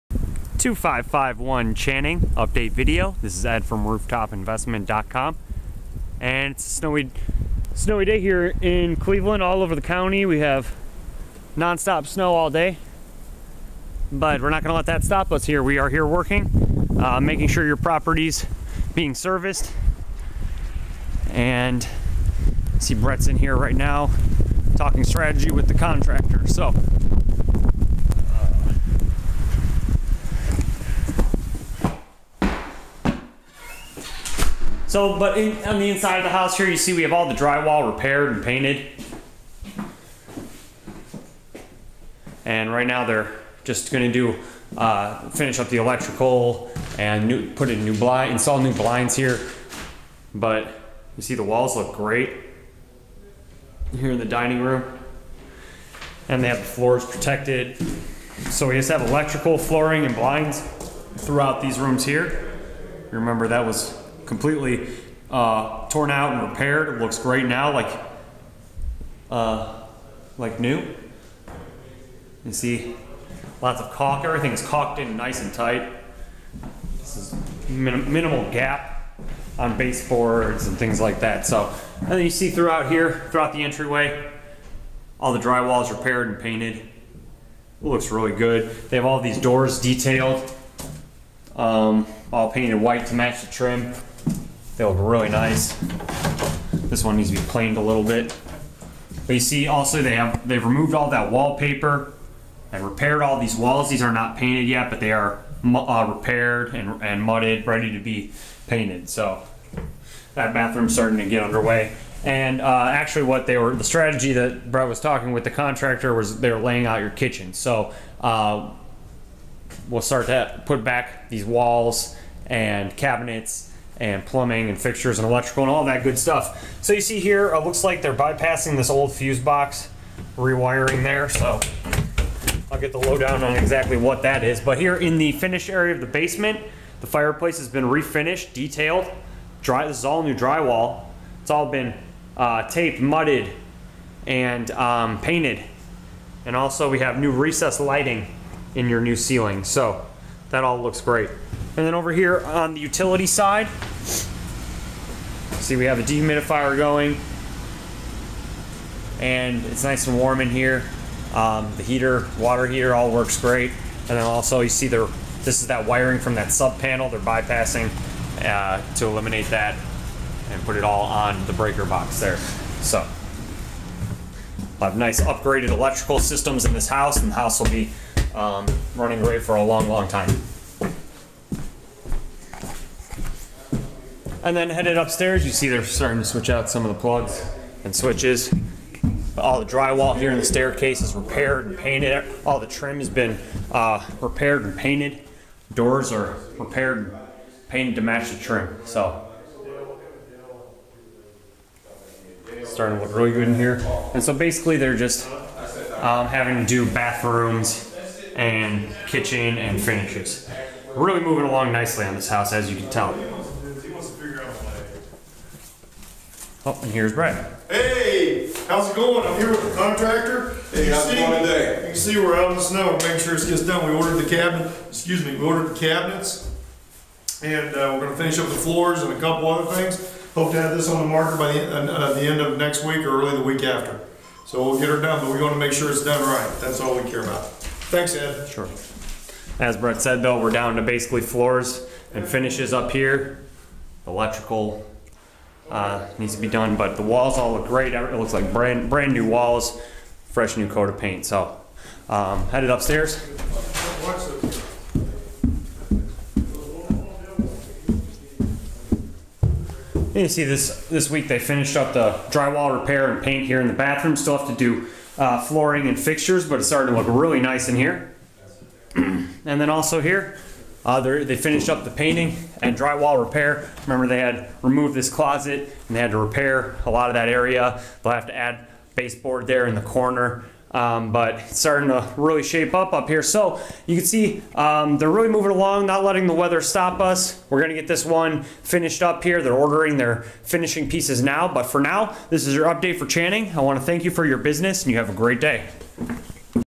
2551 Channing Update 5
This is a large 4BR 1.5BA single family home in University Heights Ohio. This home features a finished basement, new windows and a large open floor flan.  This home is being completely updated! Updates include new electrical plugs covers switches and light fixtures fresh paint new/refinished flooring and much more!